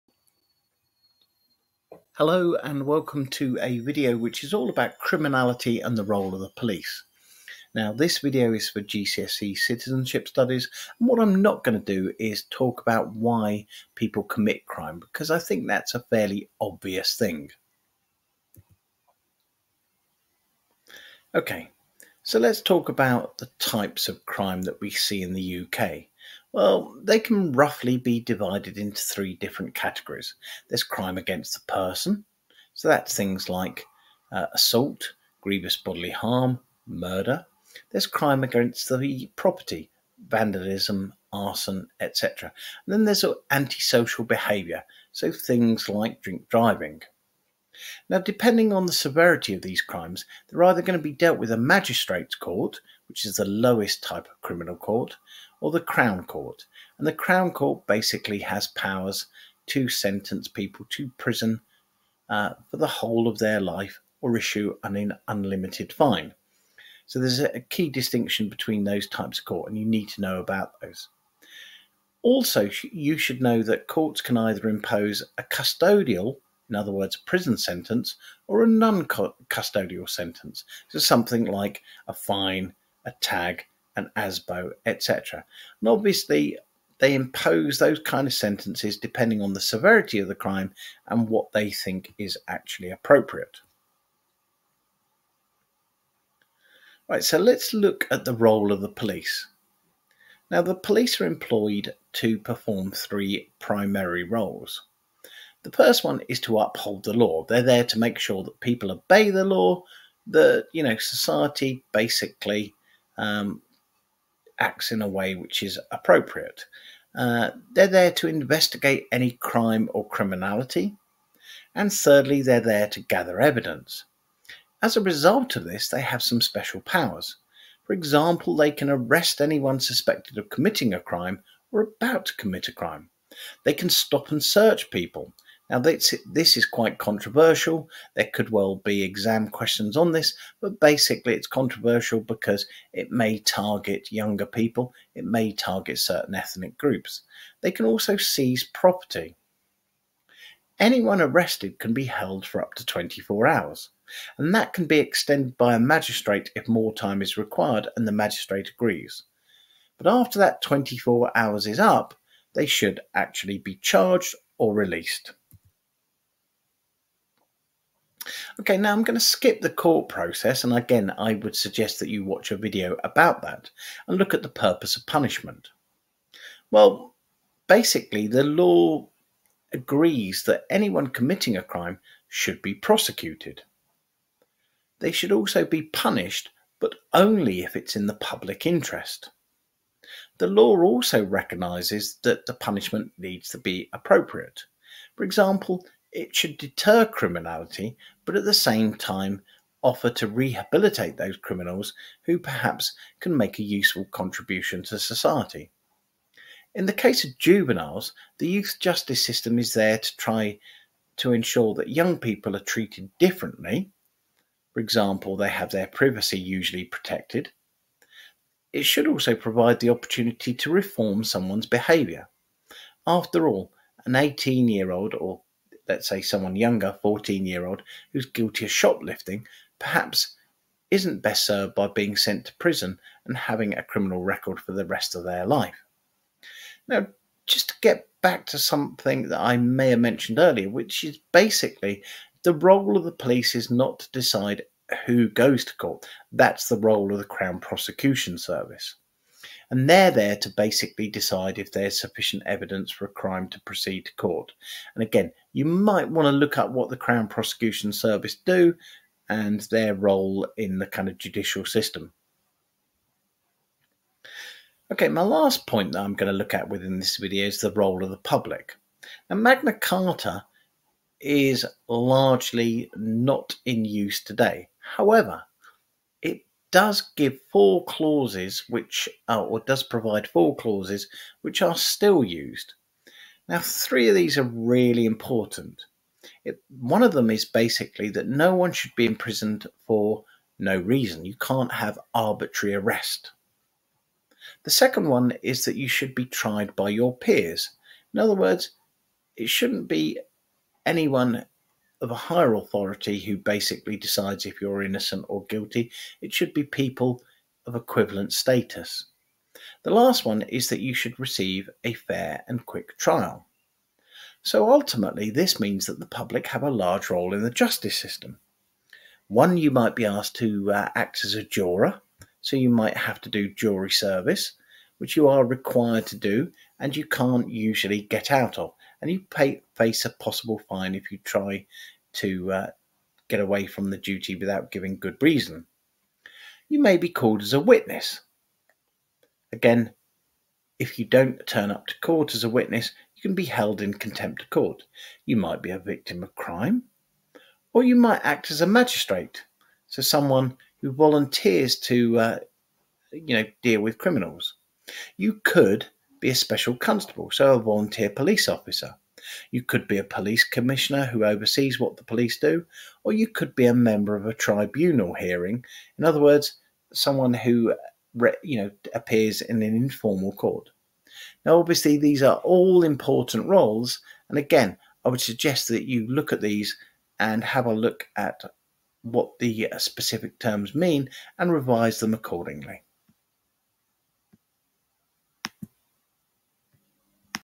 Criminality and the role of the Police
A short video for GCSE Citizenship Studies on the role of the Police and Criminality. Not my finest video, but tired at the end of a long week of A level and GCSE exams!